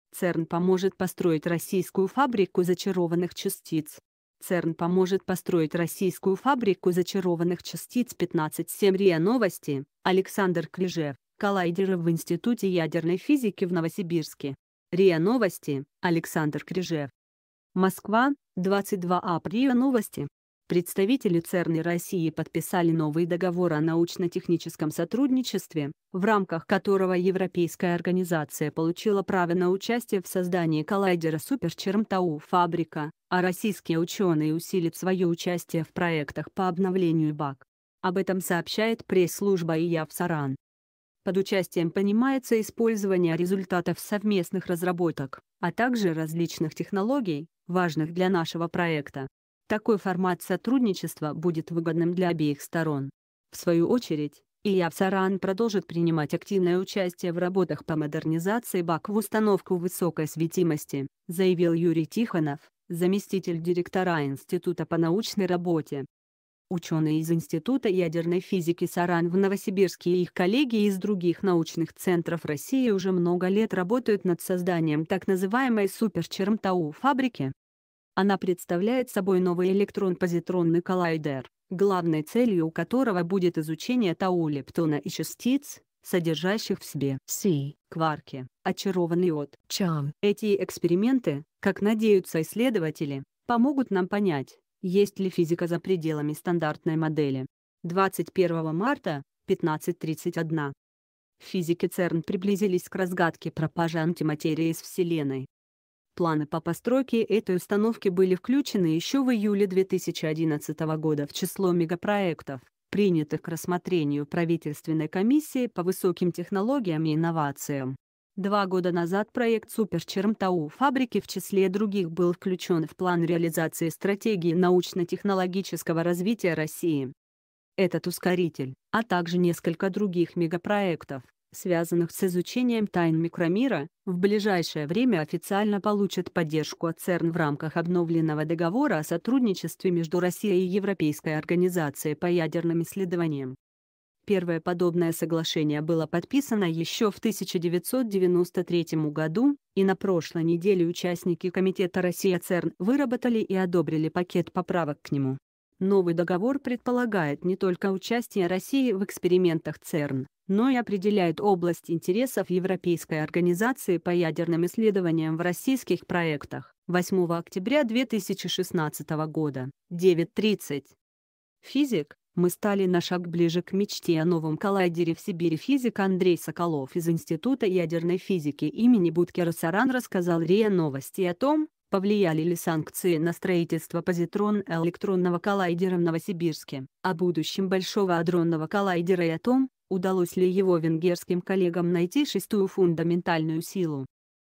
ЦЕРН поможет построить российскую фабрику зачарованных частиц
ЦЕРН поможет построить российскую фабрику зачарованных частиц ЦЕРН поможет построить российскую фабрику зачарованных частиц 15 07 РИА Новости Александр Кряжев Коллайдеры в институте ядерной физики в Новосибирске РИА Новости Александр Кряжев МОСКВА, 22 апр РИА Новости . Представители ЦЕРН и России подписали новый договор о научно-техническом сотрудничестве, в рамках которого европейская организация получила право на участие в создании коллайдера Супер Чарм-тау фабрика , а российские